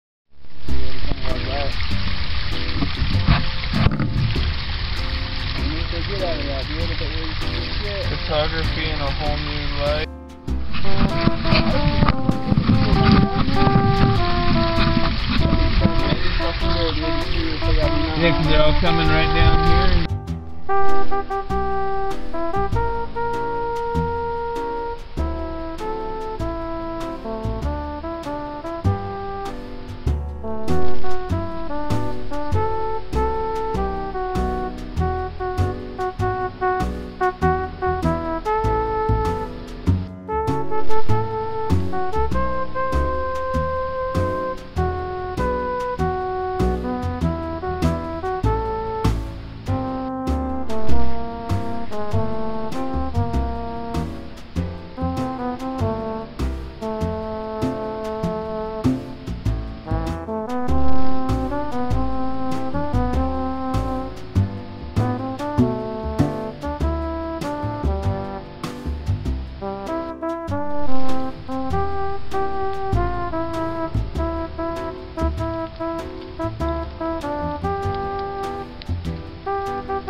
Lake San Marcos Trail Cam Feb 2017
Mostly visited by raccoons, with one visit by a coyote on a rainy night.  One visit by a human, who was curious about my business card, and hopefully was just putting it back on the camera.